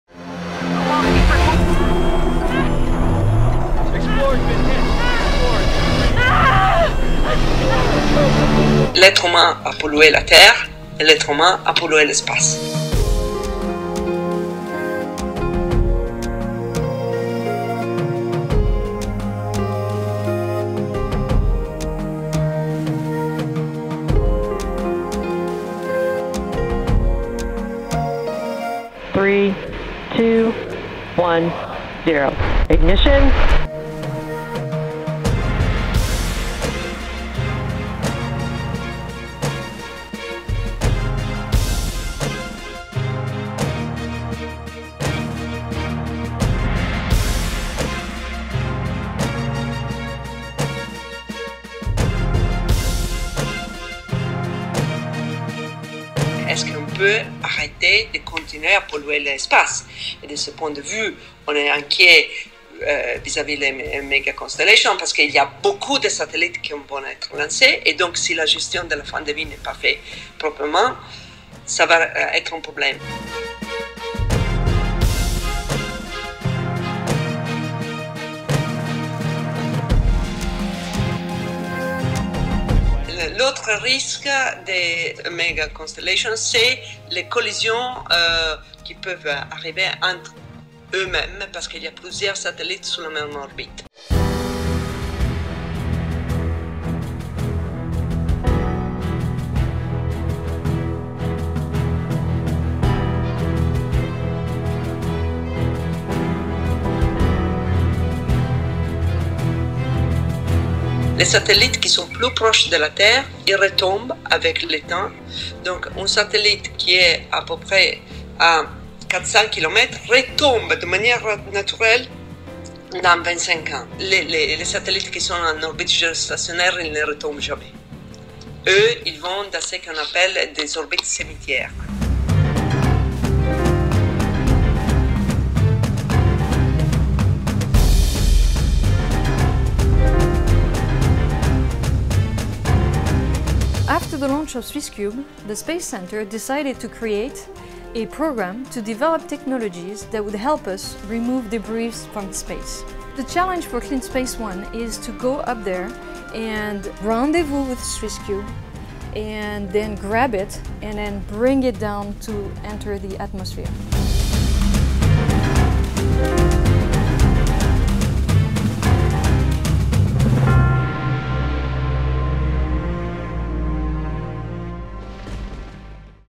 Pollution spatiale: des milliers de débris en orbite autour de la terre
SpaceX a lancé début janvier une nouvelle grappe de satellites pour forger son réseau StarLink. Ce lancement vient enrichir un peu plus un ciel déjà bien chargé d’appareils et de débris. Décryptage de la pollution spatiale en 3 minutes.